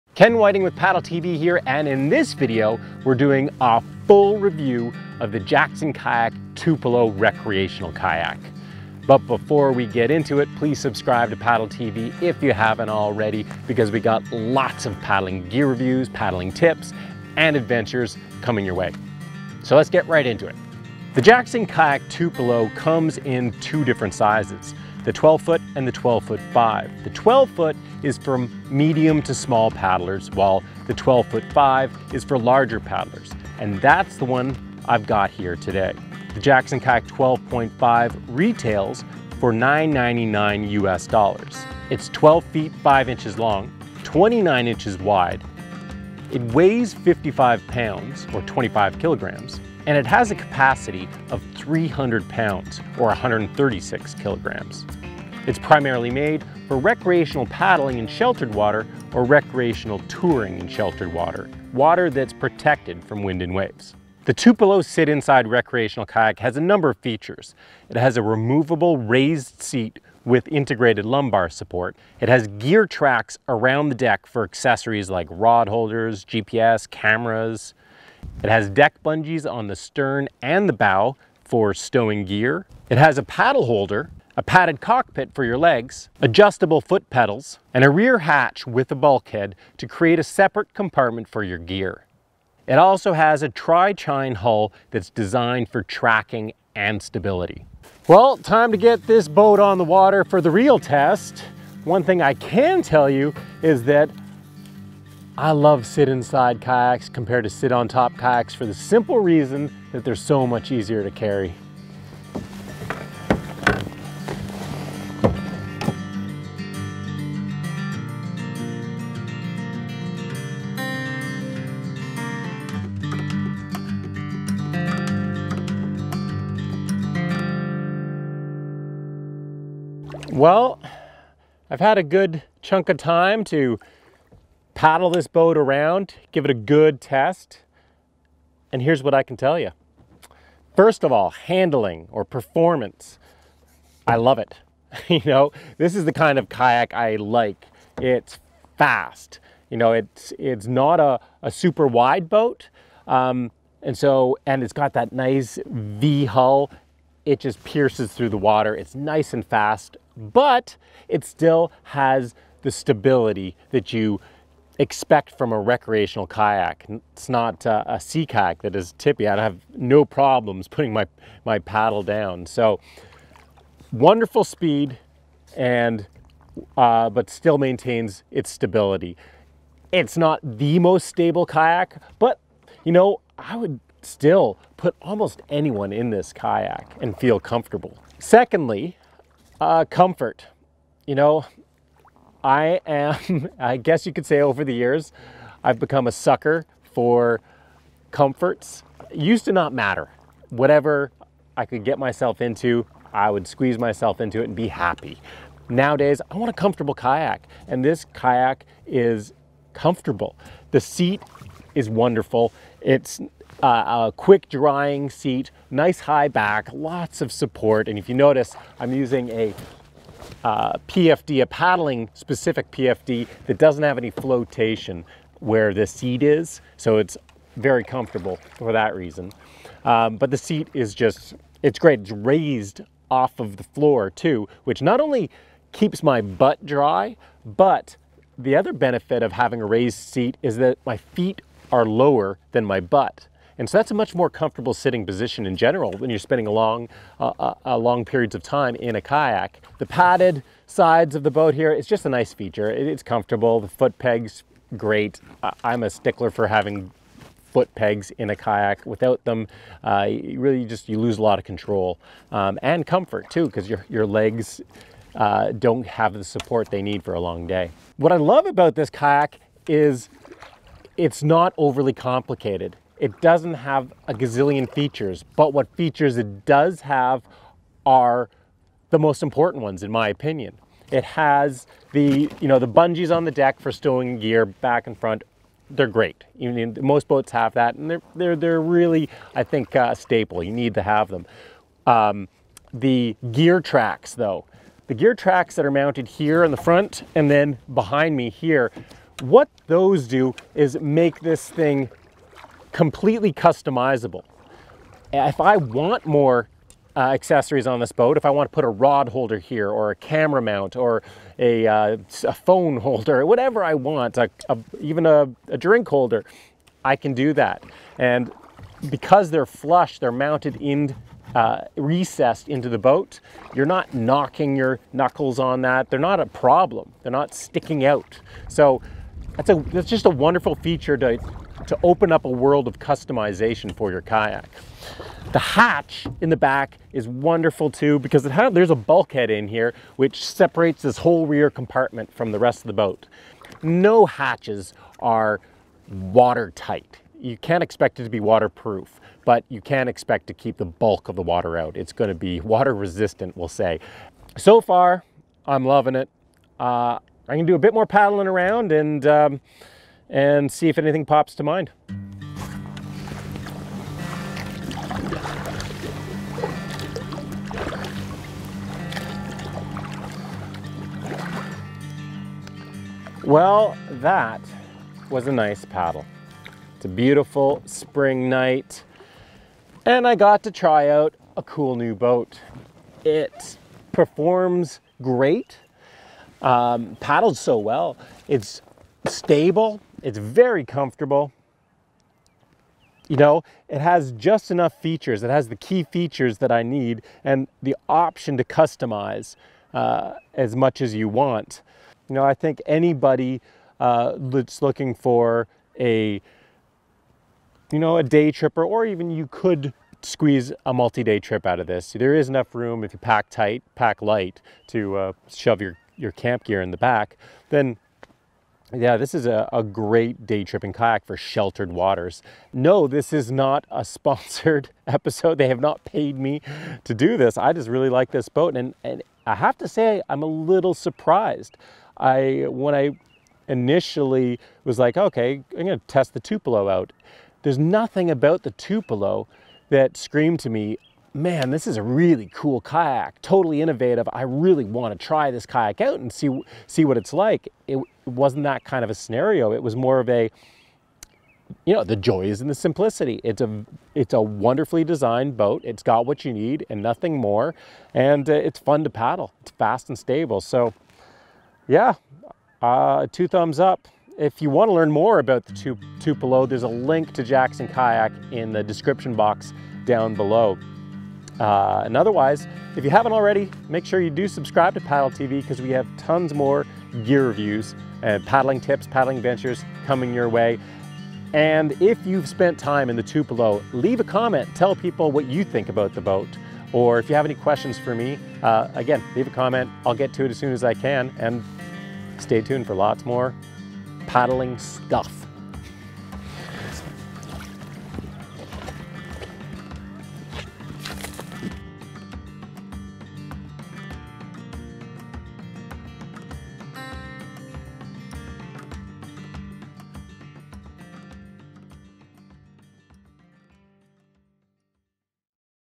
Jackson Kayak - Tupelo 12.5 | Gear Test and FULL Review
In this video I'm doing a full testing and review of the Jackson Kayak Tupelo recreational touring kayak. This kayak is in the very competitive mid-range kayak market, which includes two of the top selling kayaks of all time - the Wilderness Systems Pungo and the Old Town Loon.  How does it compare?  Is the Jackson Kayak Tupelo worth buying?  Hope you enjoy this Jackson Kayak Tupelo Review!